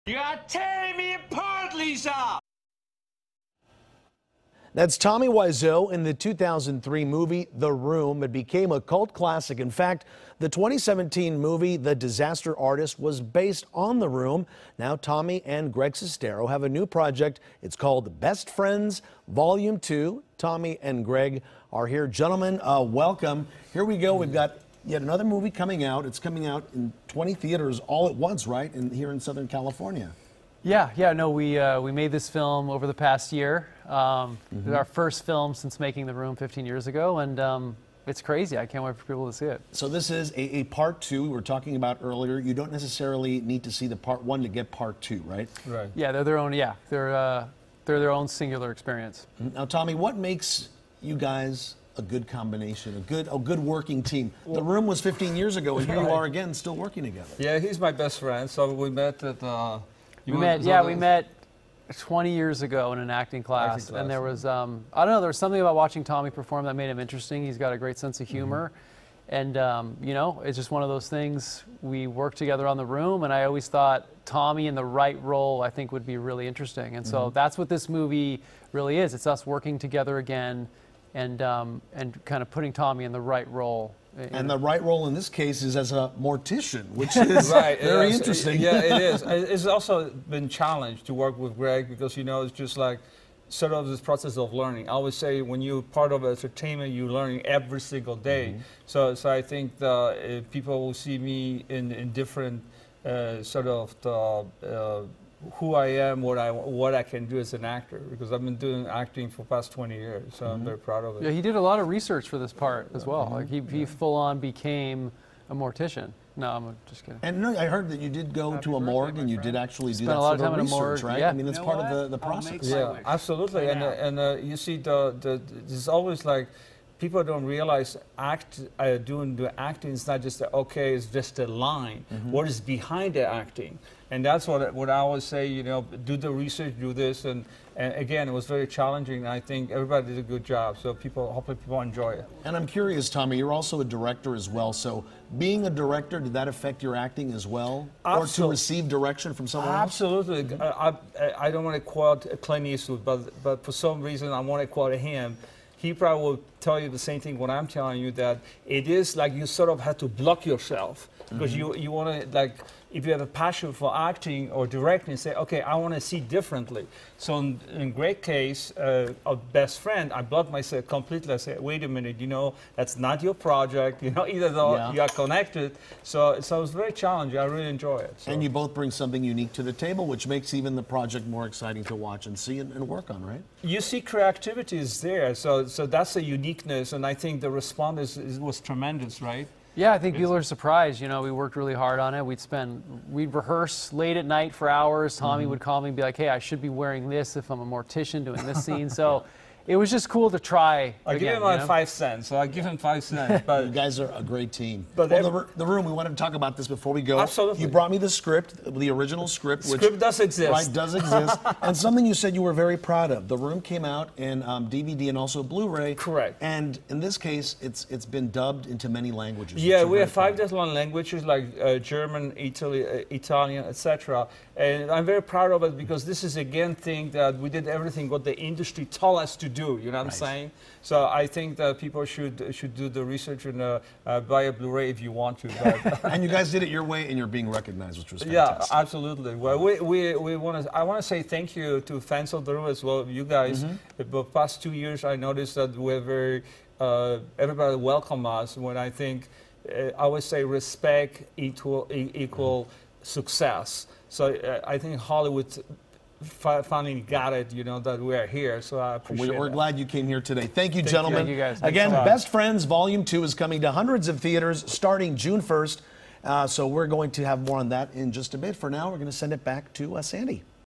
Actors Tommy Wiseau, Greg Sestero Talk 'The Room,' 'Best Friends'
Actors Tommy Wiseau and Greg Sestero, stars of the cult classic "The Room," talk about their new movie "Best Friends Volume Two."